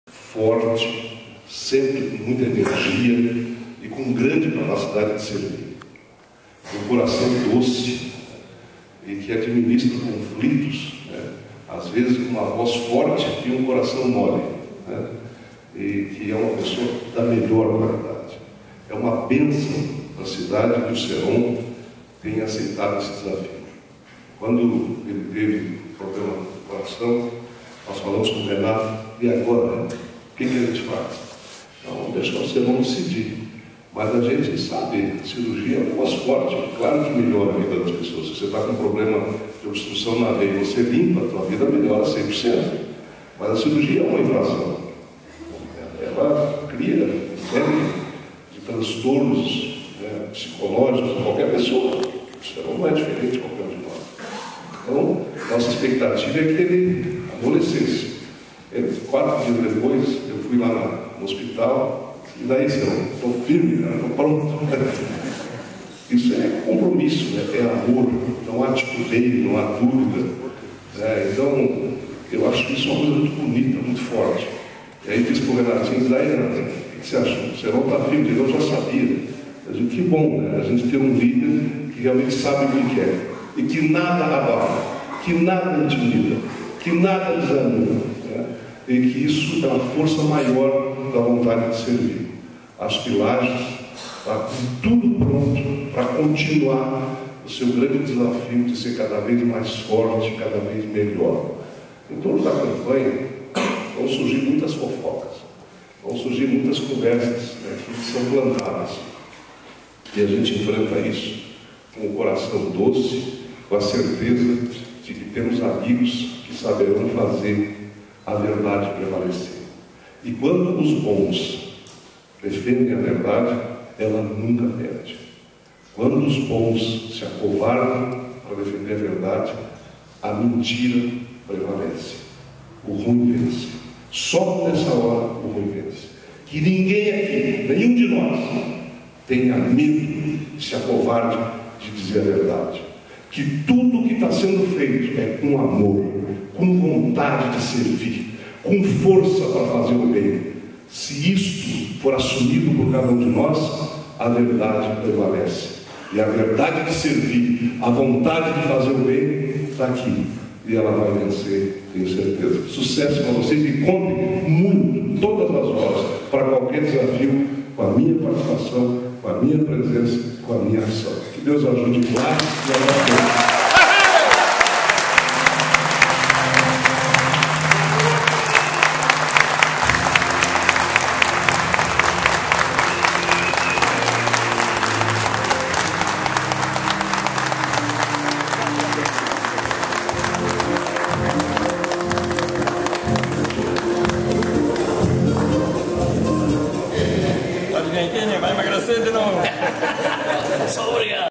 Governador Raimundo Colombo fala sobre Antonio Ceron - Junho/2012
Governador Raimundo Colombo, em Lages, fala sobre seu amigo e pré-candidato a prefeitura de Lages Antonio Ceron.